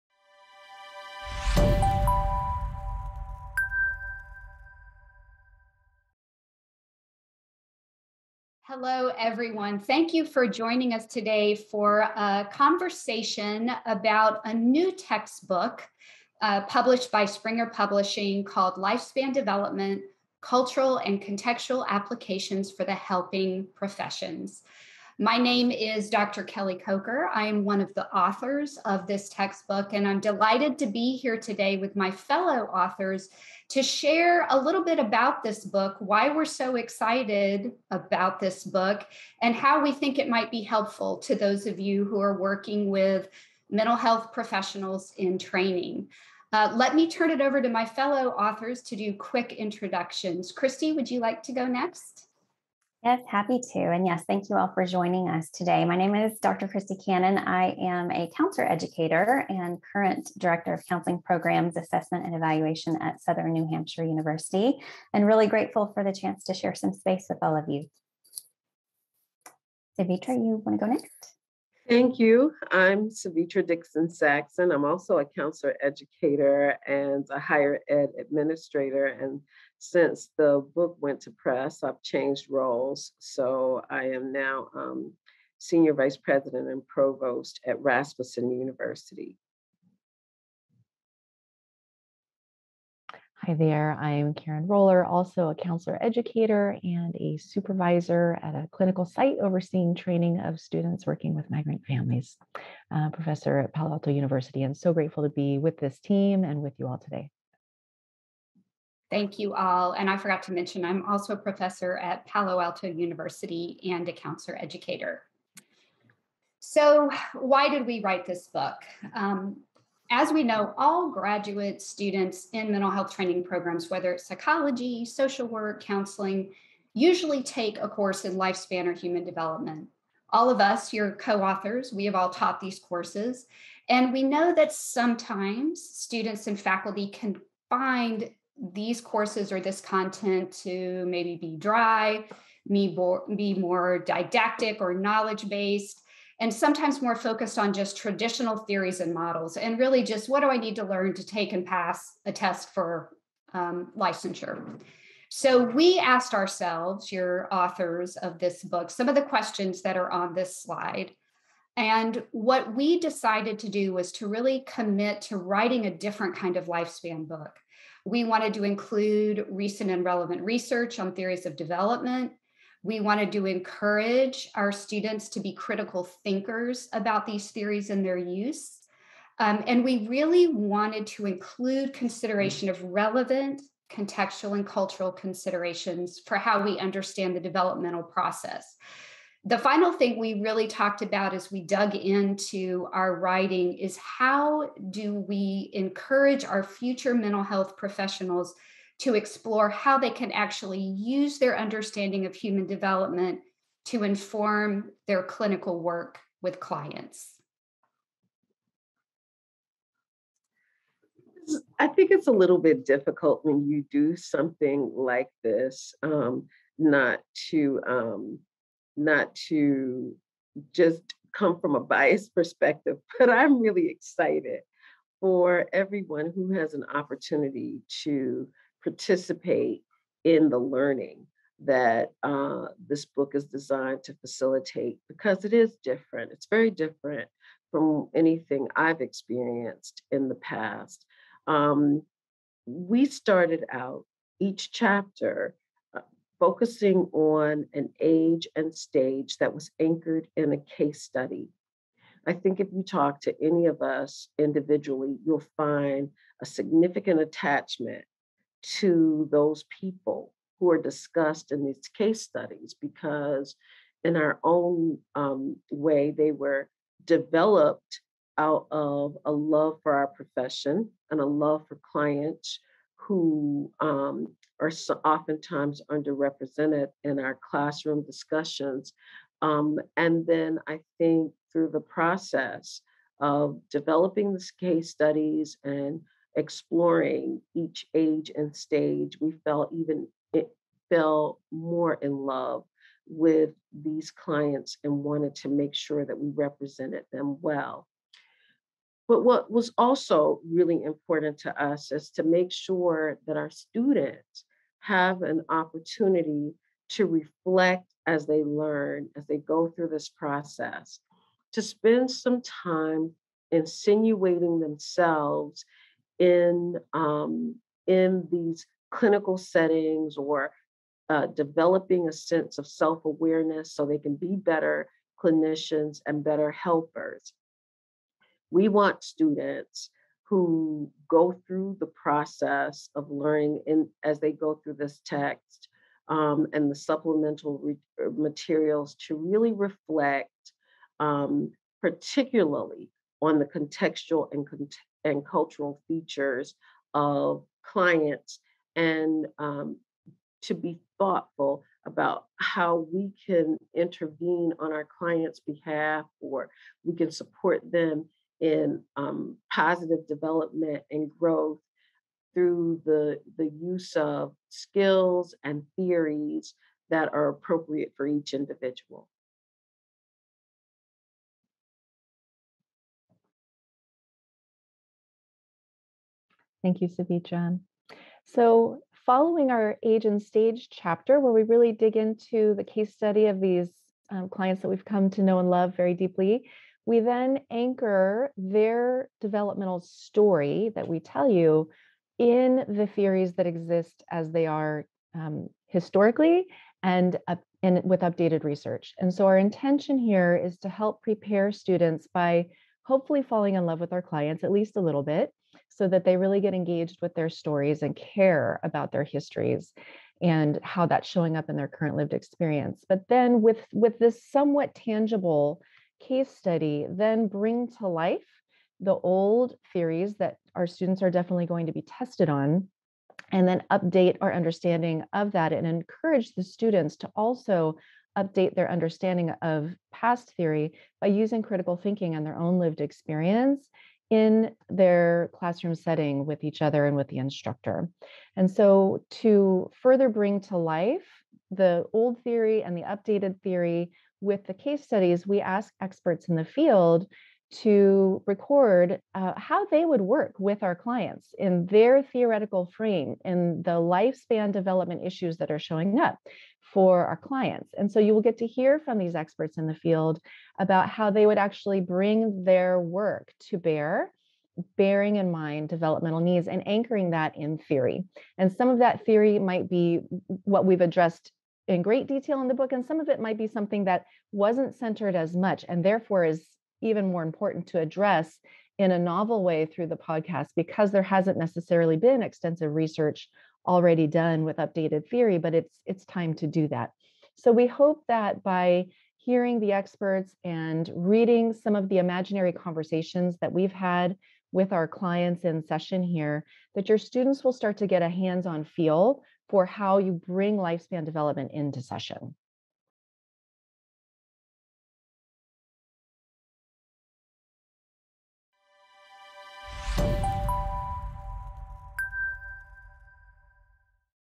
Designed to foster the transition of theory to clinical application for your students.
Learn how the authors of the brand-new Counseling text, Lifespan Development: Cultural and Contextual Applications for the Helping Professions, worked to provide students with fundamental knowledge while challenging them to question, evaluate, and consider contextual factors when applying developmental theories. for your copy today!